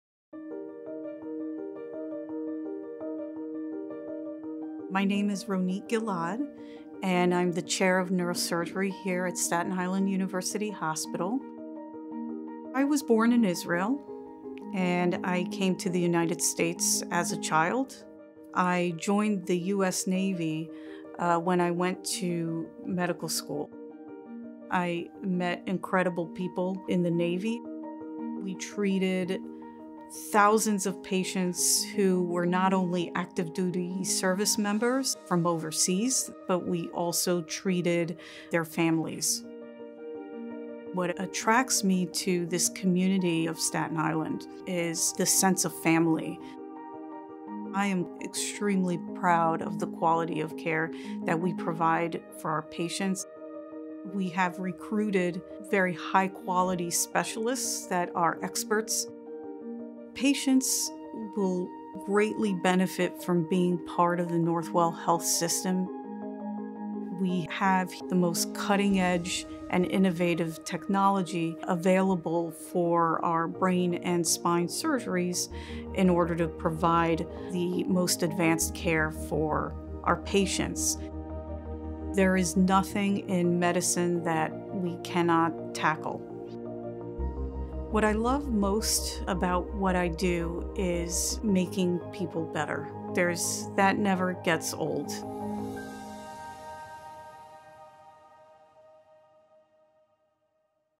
Ronit Gilad, MD
Ronit Gilad, MD, serves as chair of neurosurgery at Staten Island University Hospital (SIUH), assistant professor of neurosurgery at Donald and Barbara Zucker School of Medicine at Hofstra/Northwell, member of the Katz Institute of Women's Health, and member of the American Association of Neurological Surgeons and the Congress of Neurological Surgeons.

For information on our more than 100 medical specialties, visit Northwell.edu and follow us @NorthwellHealth on Facebook, Twitter, Instagram and LinkedIn.